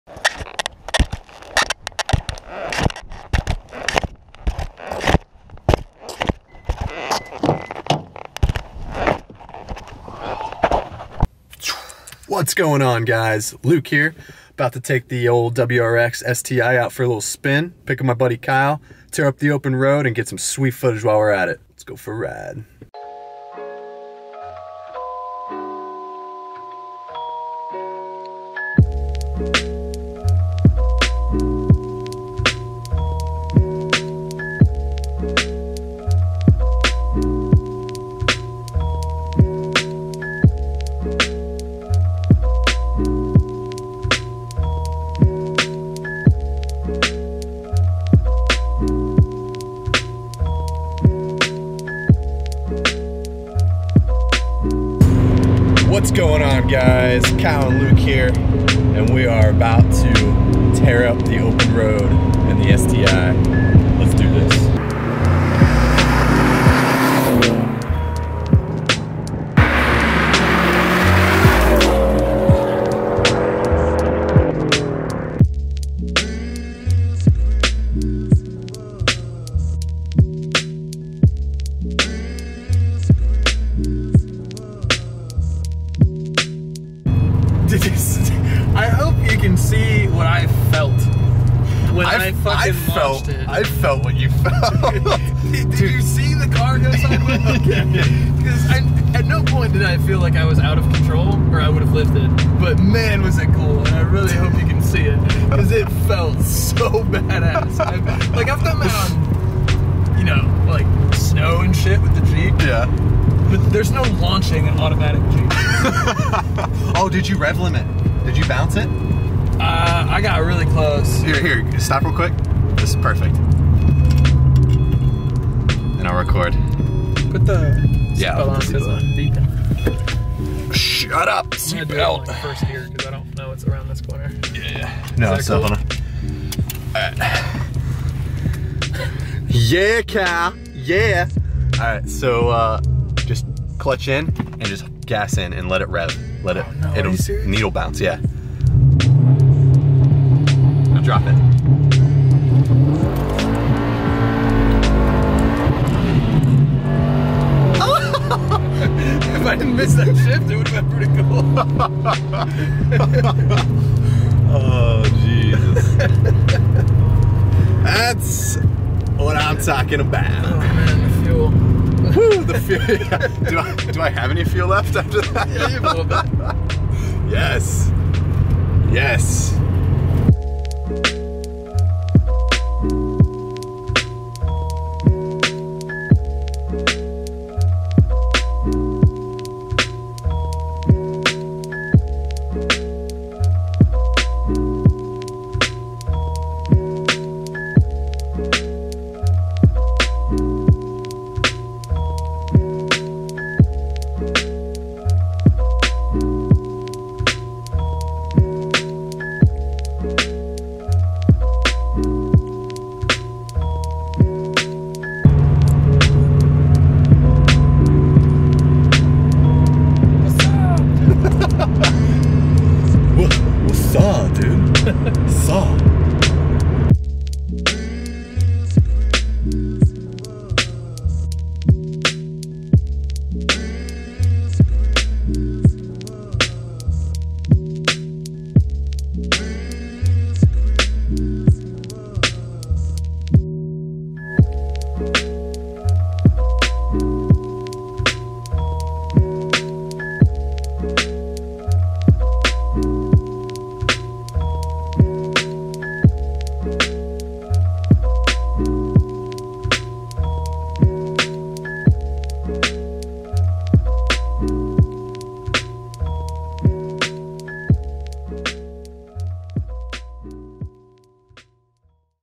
Subaru WRX STI - "The Harbor"
"The Harbor" | The cinematic "Vlog" venture of Henderson Harbor, NY.

Yesterday, I picked up my good friend, Kyle, in my 2014 WRX (STi Convert) and we decided to tear up the roads near one of my favorite places in Upper State, New York. Sit back, Relax and enjoy the boost.

Car MODS List:
2014 Subaru WRX (STi Convert)
- Brembo Brakes
- COBB SF Intake w/SF AirBox
- COBB SS Turbo Heatshield
- COBB Lightweight Main Pulley
- COBB Stage 1+ Drivetrain Pkg
- COBB Accessport V3
- STi Short Throw Shifter
- Perrin Strut Engine Brace
- Perrin Sport Lowering Springs
- Process West Verticooler Top Mount Intercooler
- ProTune (AIG Performance - Maryland, PA)

- Luca Joseph